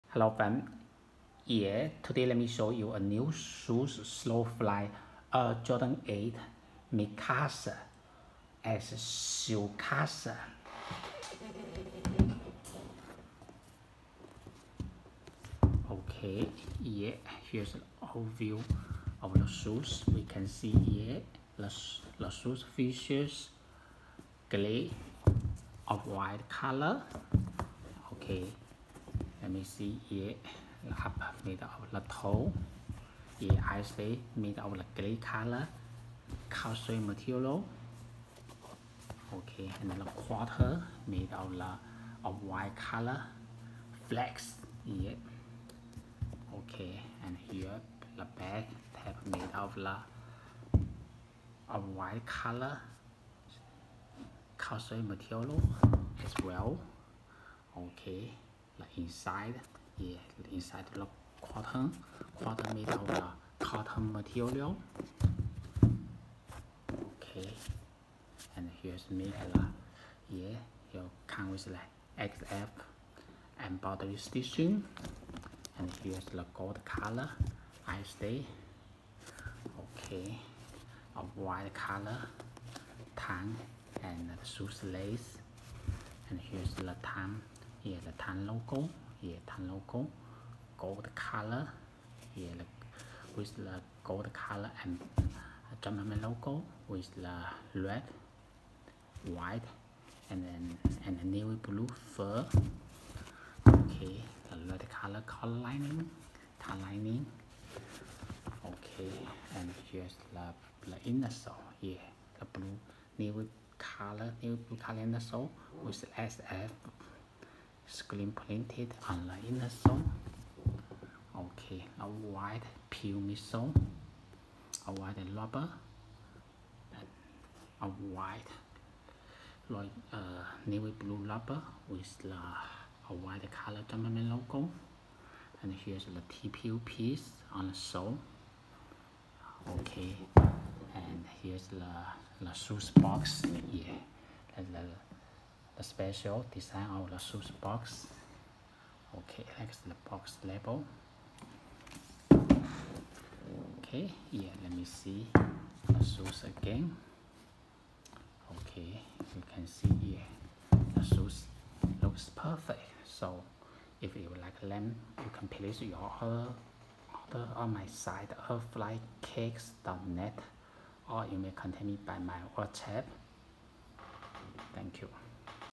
SoleFly x Air Jordan 8 Mi Casa Es Su Casa Review
SoleFly x Air Jordan 8 Mi Casa Es Su Casa

Hey friend today give you the 1st detailed review on SoleFly x Air Jordan 8 Mi Casa Es Su Casa,if you like them,pls click the below link to order on my

The Jordan 8 Retro SP SoleFly Mi Casa Es Su Casa represents a distinctive fusion of style and heritage, drawing inspiration from the Bandera de Cuba to celebrate Cuban culture.

With a sail upper providing a neutral canvas, the SoleFly Mi Casa Es Su Casa Jordan 8 Retro SP design comes to life with vibrant colors, where golden accents introduce an element of elegance and red details infuse energy into the overall aesthetic. Exclusive branding elements like the SoleFly hangtag and lace tips contribute to the shoe's uniqueness. The chenille Jumpman tongue logo, a signature feature, stands prominently amid the rich blend of Sail and Fossil neutrals. Metallic Gold accents add a further layer of sophistication, concluding the design with an elegant touch.

The standout characteristic of the Jordan 8 Retro SP SoleFly Mi Casa Es Su Casa lies in its captivating color scheme. Drawing inspiration from the Bandera de Cuba, these colors breathe vitality into the shoe, crafting a vibrant and meaningful tribute to Cuban heritage. The sneaker debuted for $225 on December 16th, 2023.